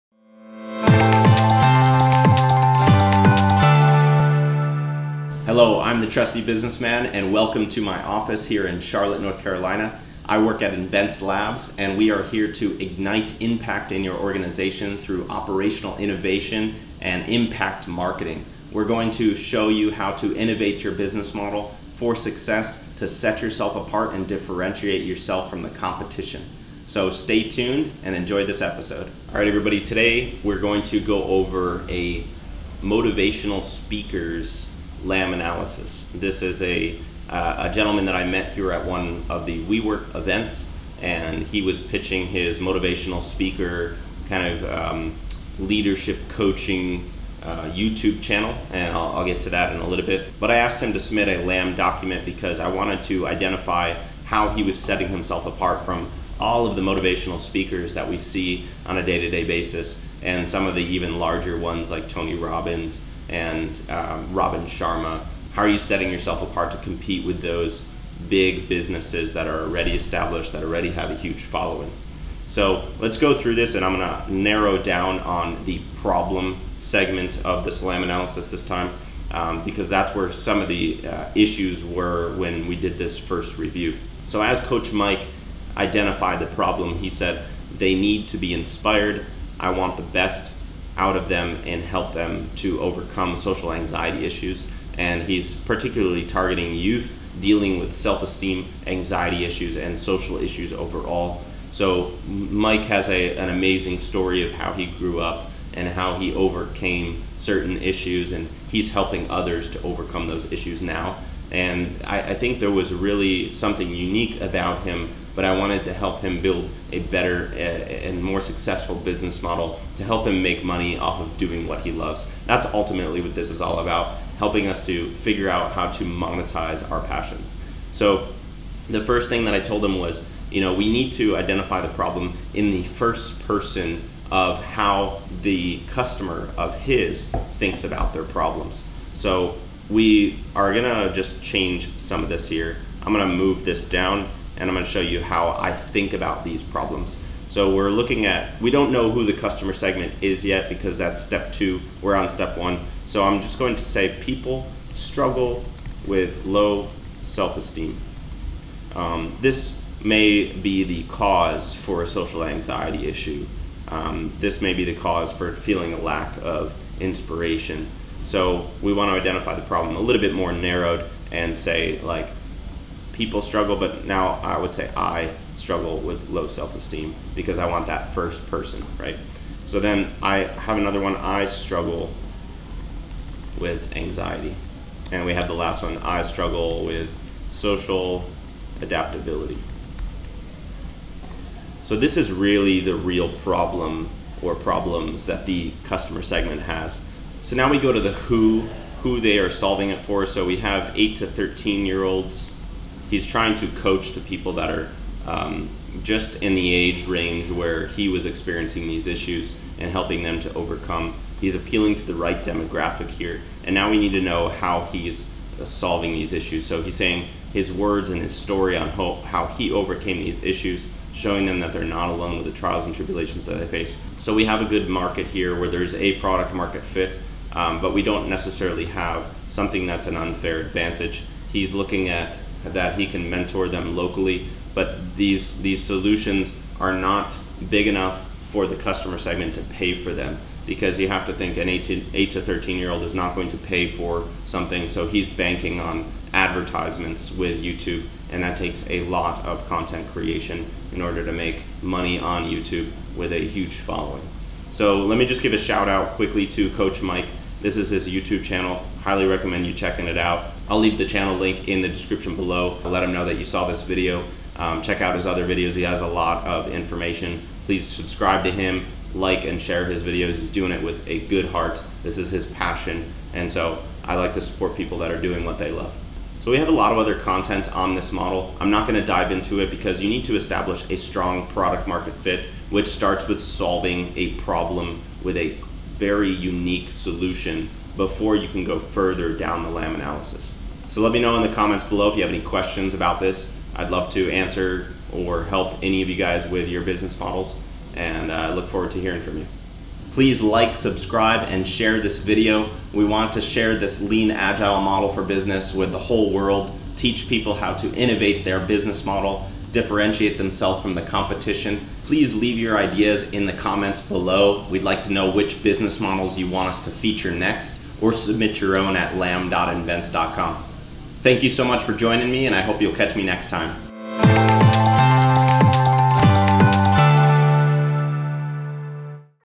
Validating a Motivational Speaker Business
Coach Myke is passionate about helping youth overcome low self-esteem and social adaptability issues by coaching and inspiring them through his personal life experiences. He's exclusively aimed at instilling confidence in youth by showing them that they're not alone in the social anxiety issues they face.

The Lean Agile Model for Business is a shortcut to writing a business plan and helps identify Product-Market Fit, Go-to-Market Strategy and Operational Innovation to build a successful business in a competitive marketing.